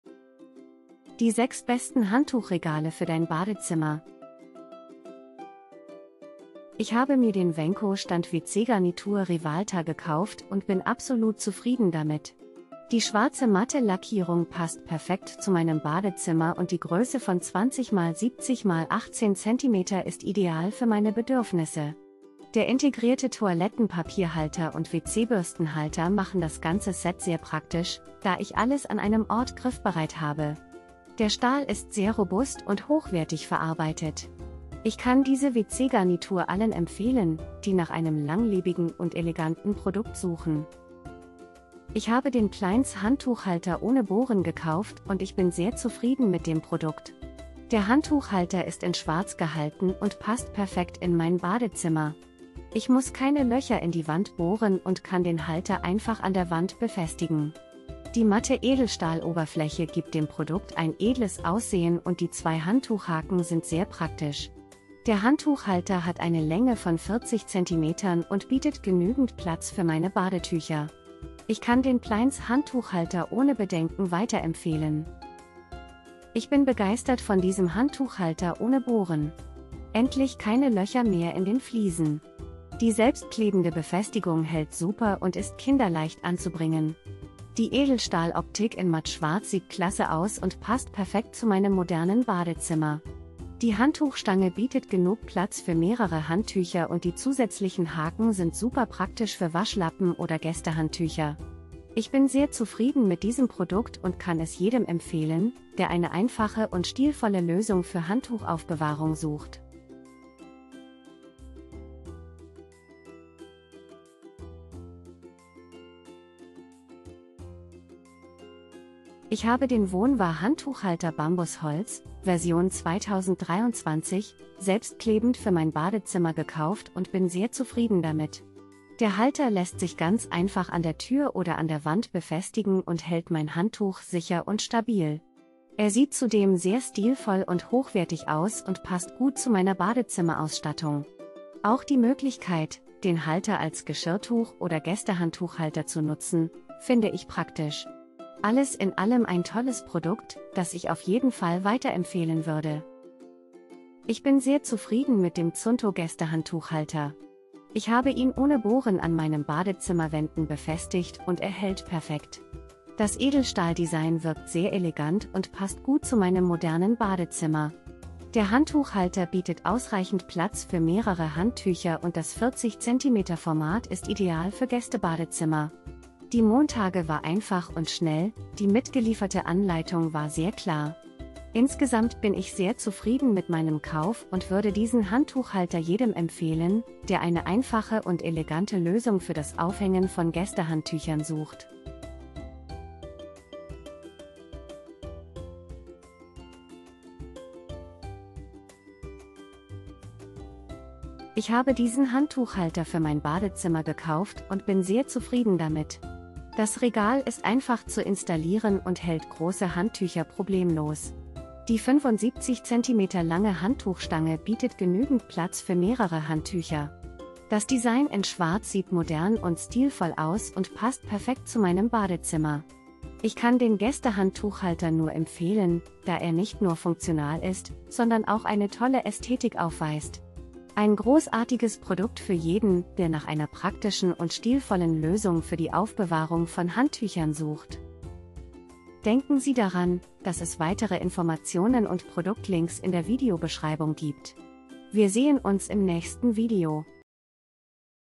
HANDTUCHREGAL BADEZIMMER: Die 6 besten Handtuchregale für dein Badezimmer
✋  Hier sind die 6 besten handtuchregale für dein badezimmer auf dem Markt.

🔽🔽🔽 KAUFEN-LINKS HANDTUCHREGAL BADEZIMMER 🔽🔽🔽

1) WENKO Stand WC-Garnitur Rivalta, mit integriertem Toilettenpapierhalter und WC-Bürstenhalter, lackierter Stahl, 20 x 70 x 18 cm, Schwarz matt

2) plainshe Handtuchhalter Ohne Bohren, Handtuchhalter Schwarz, Handtuchhalter Bad, GäStehandtuchhalter, Handtuchhalter Wand Matt Edelstahl, Badetuchhalter. (40cm，Mit 2 StüCk Handtuchhaken)

3) Handtuchhalter Ohne Bohren, Gästehandtuchhalter Selbstklebend, Edelstahl Handtuchstange Wandmontage mit 2 Handtuchhaken für Badezimmer und Küche (40cm,Matt Schwarz)

4) WOHNWAHR® Handtuchhalter Bambus Holz [Version 2023] selbstklebend Handtuchhalter ohne Bohren für Bad Badezimmer Tür Küche Gäste-WC | Geschirrtuch-Halter Gästehandtuchhalter

5) ZUNTO Gästehandtuchhalter 40cm Selbstklebend Ohne Bohren Handtuchhalter Edelstahl Handtuchregal Badezimmer

6) Handtuchhalter, Handtuchregal Schwarz Wand für Badezimmer gäste wc, Gästehandtuchhalter für Große Handtücher, Handtuchstange 75cm, Handtücher Aufbewahrung

* Zeitindikatoren (Timestamps) *

00:00 - Intro
00:05 - 1) WENKO Stand WC-Garnitur Rivalta, mit integriertem...
00:40 - 2) plainshe Handtuchhalter Ohne Bohren, Handtuchhalter Schwarz,...
01:15 - 3) Handtuchhalter Ohne Bohren, Gästehandtuchhalter...
01:58 - 4) WOHNWAHR® Handtuchhalter Bambus Holz [Version 2023]...
02:32 - 5) ZUNTO Gästehandtuchhalter 40cm Selbstklebend Ohne Bohren...
03:20 - 6) Handtuchhalter, Handtuchregal Schwarz Wand für Badezimmer...
03:57 - Outro